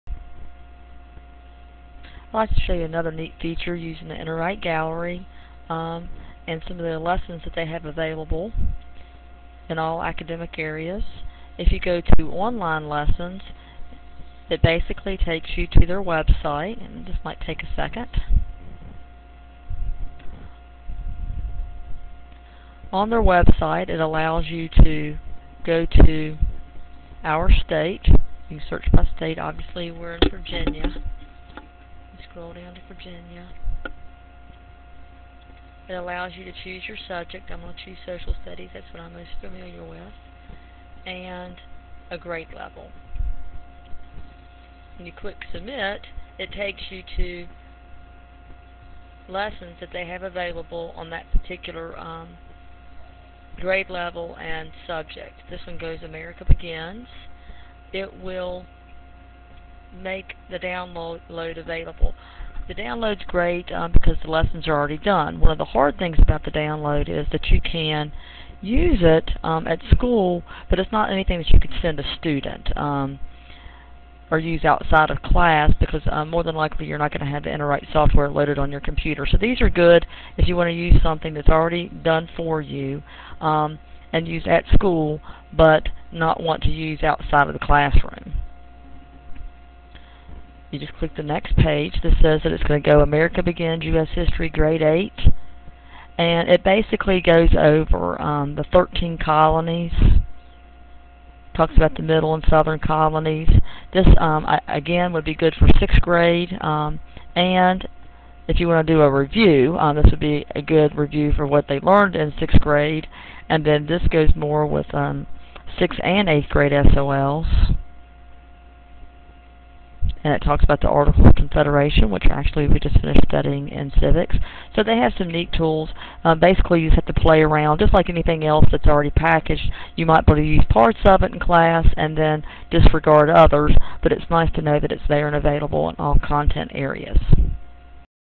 Interwrite Pad-Online Lessons
Demonstration on how to access lesson plans available on Interwrite Learning site.